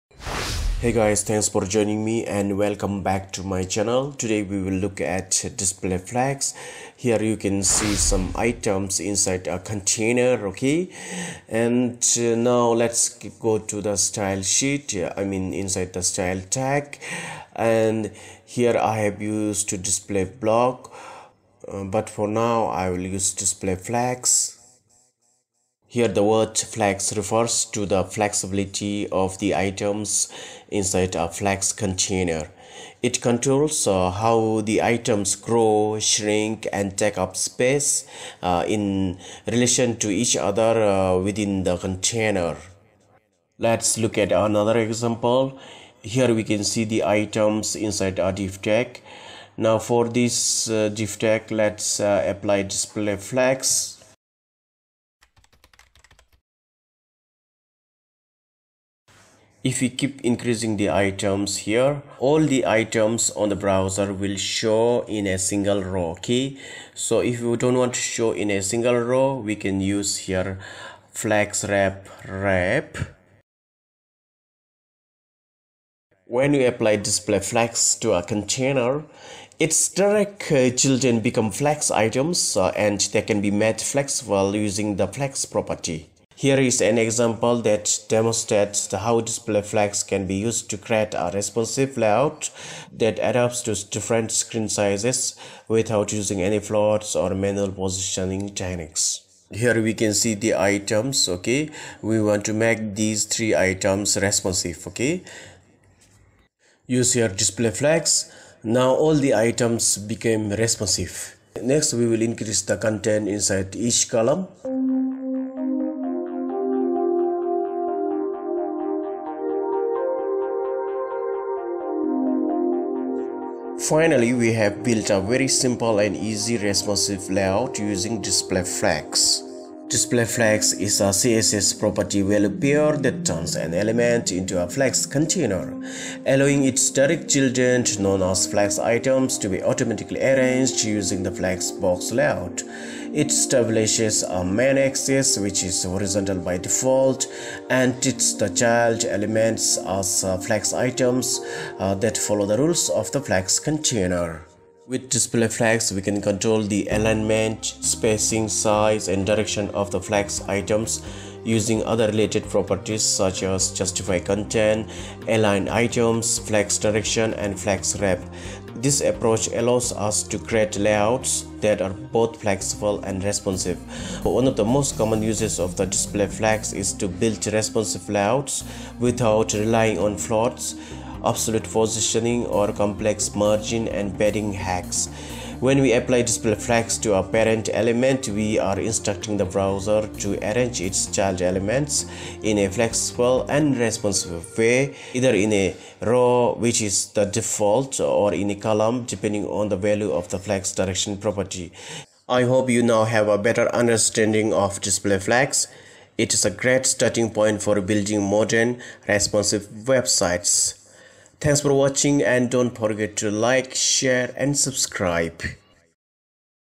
Learn CSS Display Flex in 3 Minutes 36 Seconds
One of the most common uses of display: flex; is to build responsive layouts without relying on floats, absolute positioning, or complex margin and padding hacks. When we apply display: flex; to a parent element, we are instructing the browser to arrange its child elements in a flexible and responsive way, either in a row (which is the default) or in a column, depending on the value of the flex-direction property.

📘 Recommended resource:
Developing Multi-Platform Apps with Visual Studio Code
(For learners who want to go beyond basics)